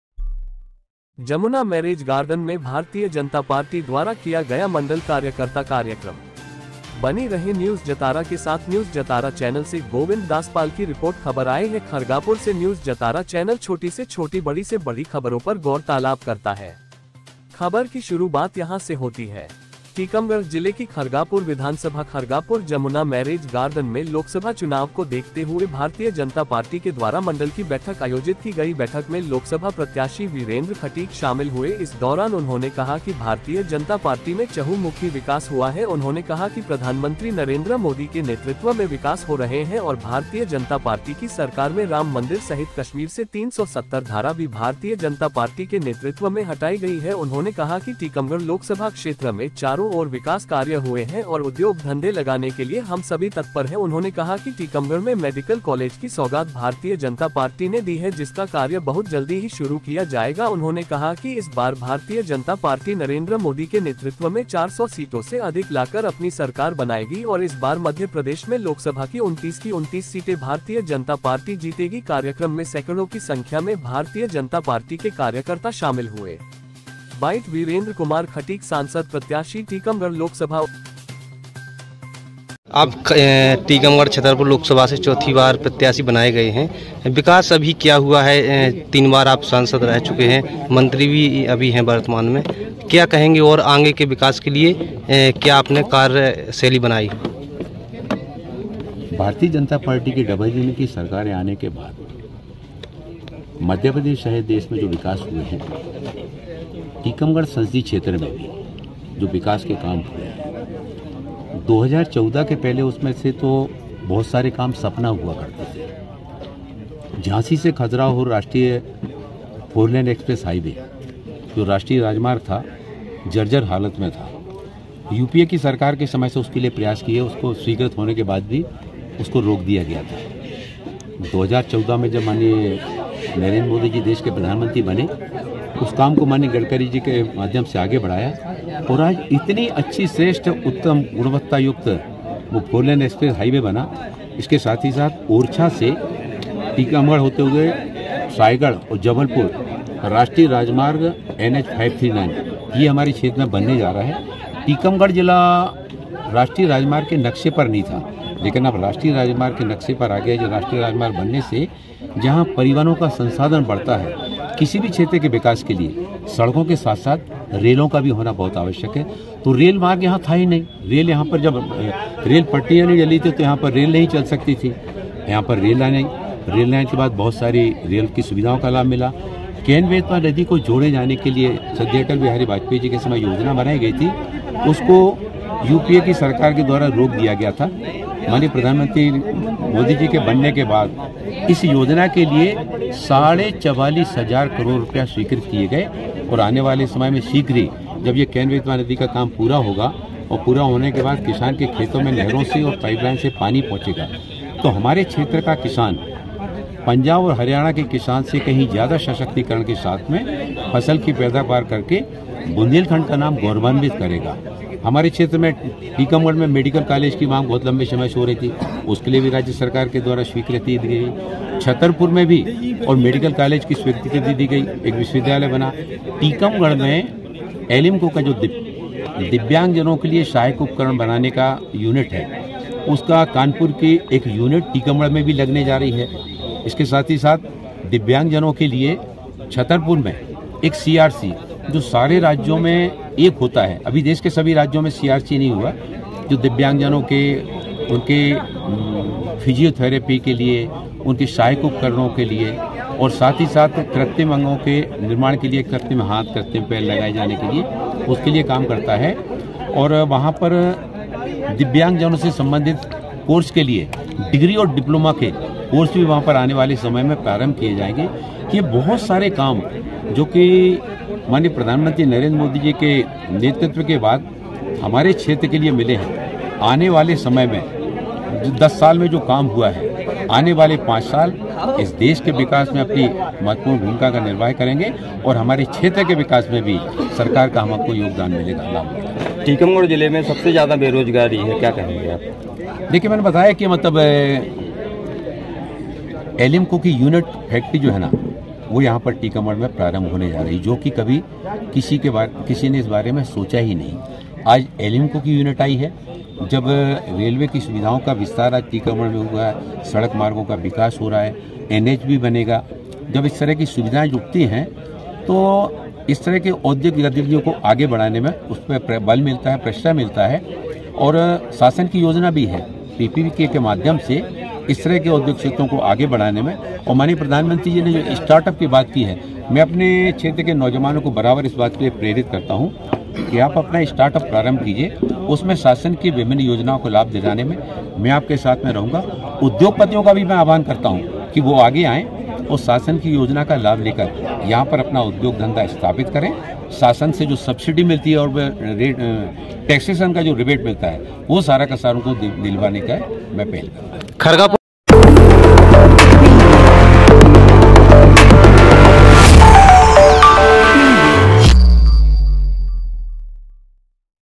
जमुना मैरिज गार्डन में भारतीय जनता पार्टी द्वारा किया गया मंडल कार्यकर्ता कार्यक्रम
टीकमगढ़ मध्य प्रदेश
खरगापुर विधानसभा
स्थान...खरगापुर
रिपोर्ट.. गोविंद दास पाल

हैडलाइन...जमुना मैरिज गार्डन में भारतीय जनता पार्टी द्वारा किया गया मंडल कार्यकर्ता कार्यक्रम...

एंकर... टीकमगढ़ जिले की खरगापुर विधानसभा खरगापुर जमुना मैरिज गार्डन में लोकसभा चुनाव को देखते हुए भारतीय जनता पार्टी के द्वारा मंडल की बैठक आयोजित की गई बैठक में लोकसभा प्रत्याशी वीरेंद्र खटीक शामिल हुए इस दौरान उन्होंने कहा कि भारतीय जनता पार्टी में चहु मुखी विकास हुआ है उन्होंने कहा कि प्रधानमंत्री नरेंद्र मोदी के नेतृत्व में विकास हो रहे हैं और भारतीय जनता पार्टी की सरकार में राम मंदिर सहित कश्मीर से 370 धारा भी भारतीय जनता पार्टी के नेतृत्व में हटाई गई है उन्होंने कहा कि टीकमगढ़ लोकसभा क्षेत्र में चारों ओर विकास कार्य हुए हैं और उद्योग धंधे लगाने के लिए हम सभी तत्पर हैं उन्होंने कहा कि टीकमगढ़ में मेडिकल कॉलेज की सौगात भारतीय जनता पार्टी ने दी है जिसका कार्य बहुत जल्दी ही शुरू किया जाएगा उन्होंने कहा कि इस बार भारतीय जनता पार्टी नरेंद्र मोदी के नेतृत्व में 400 सीटों से अधिक लाकर अपनी सरकार बनाएगी और इस बार मध्य प्रदेश में लोकसभा की 29 की 29  सीटे भारतीय जनता पार्टी जीतेगी कार्यक्रम में सैकड़ो की संख्या में भारतीय जनता पार्टी के कार्यकर्ता शामिल हुए...

बाइट...वीरेंद्र कुमार खटीक सांसद प्रत्याशी टीकमगढ़ लोकसभा

आज तक 24/7 न्यूज़ से जिला